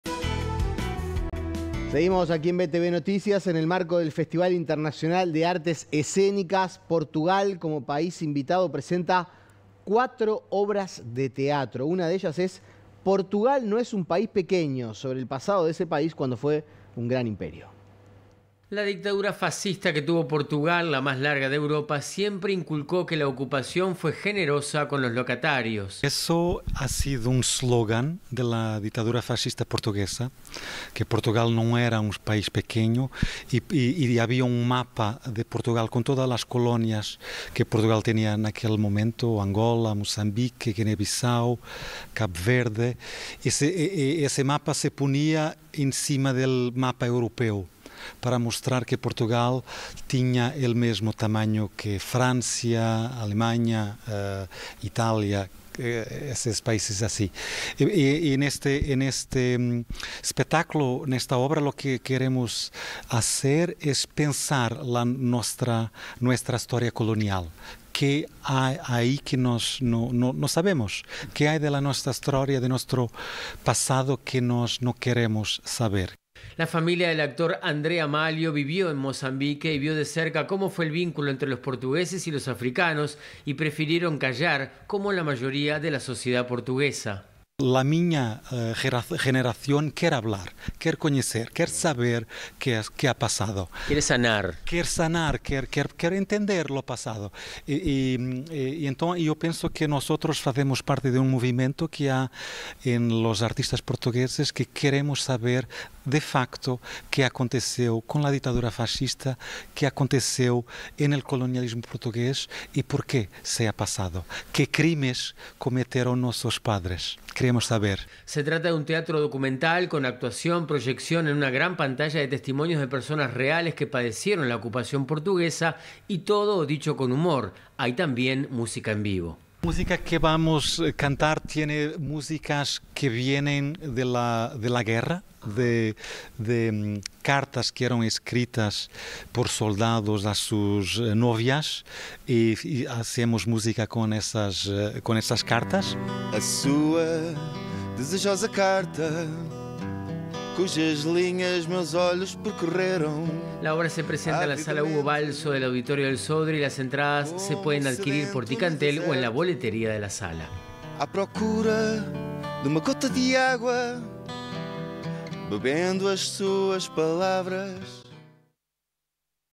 Edición Central 22/08 | Portugal estará presente en el Festival Internacional de Artes Escénicas
En el marco del Festival Internacional de Artes Escénicas, Portugal como país invitado presenta cuatro obras de teatro.

Una de ellas es “Portugal no es un país pequeño”, sobre el pasado de ese país cuando fue un gran imperio.